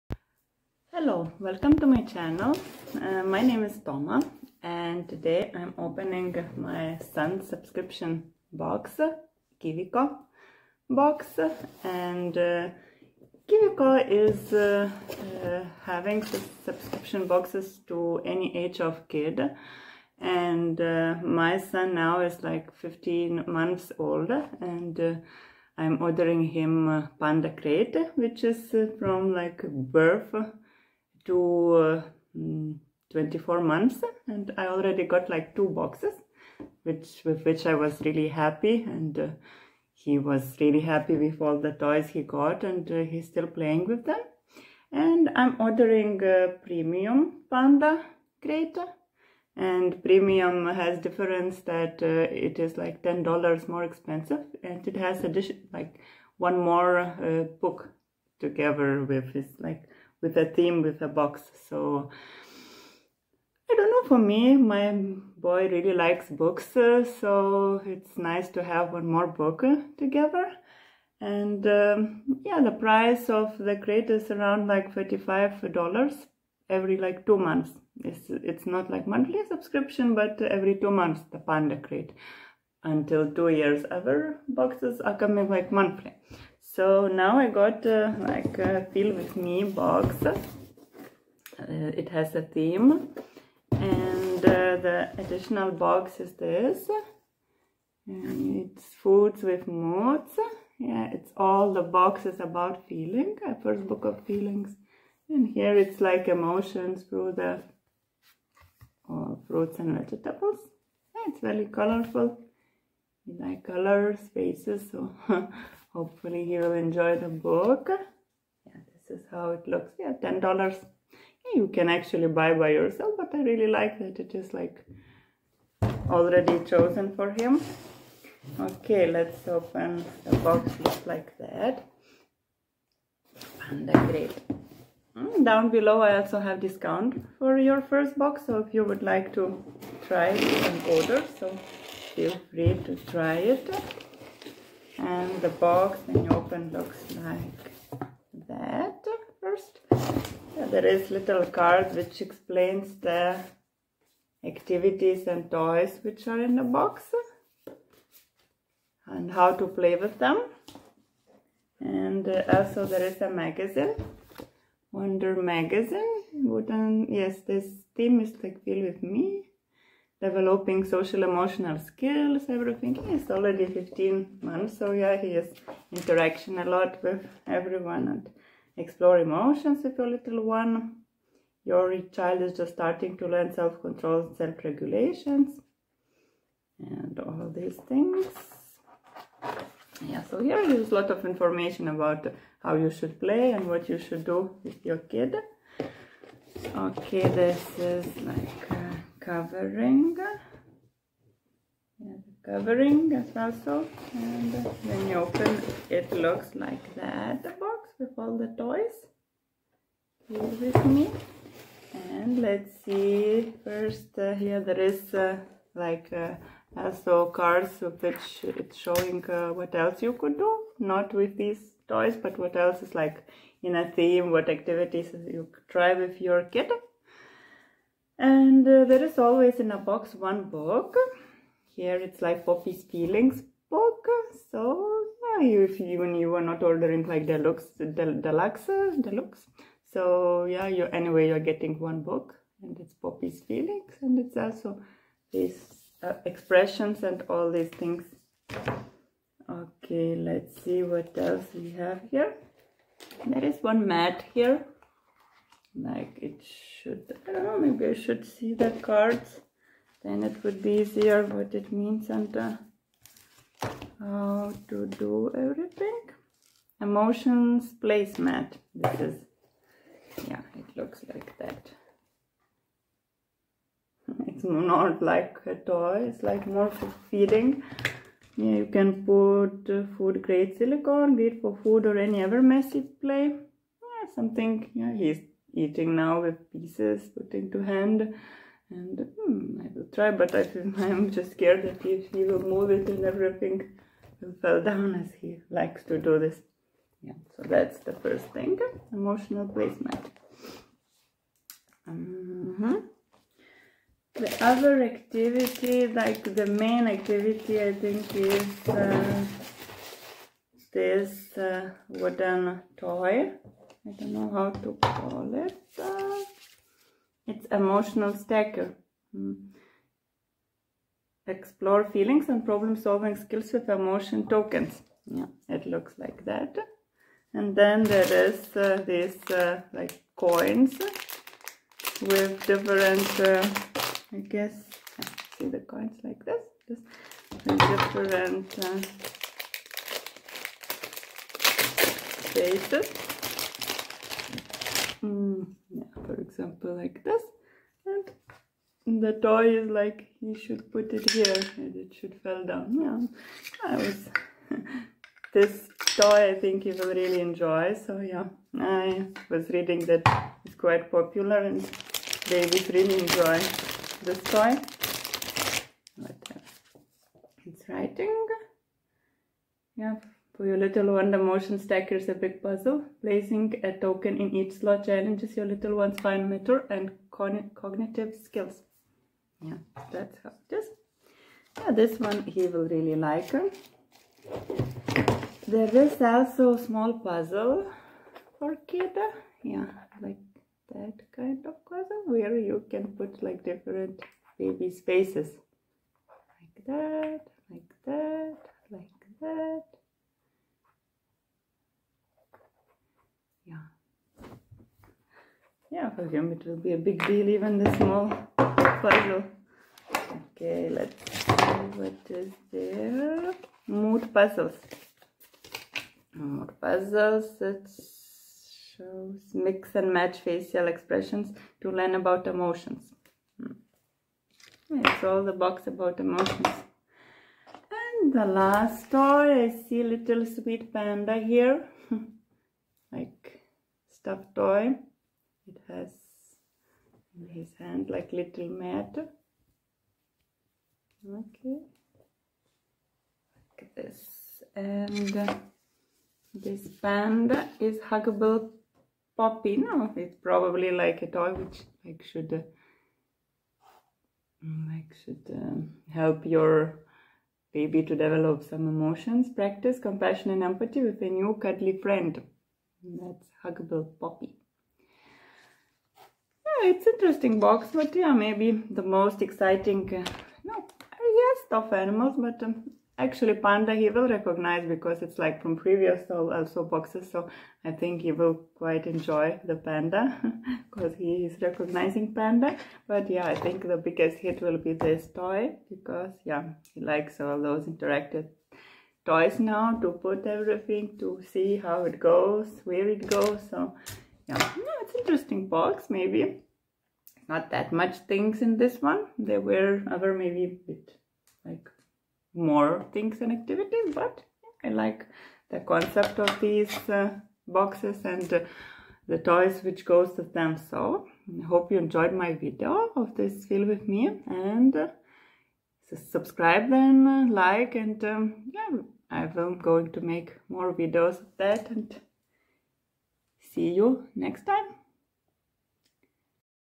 Kiwico Premium Panda crate "Feel with me", 50% off
Opening Panda crate and my opinion about it.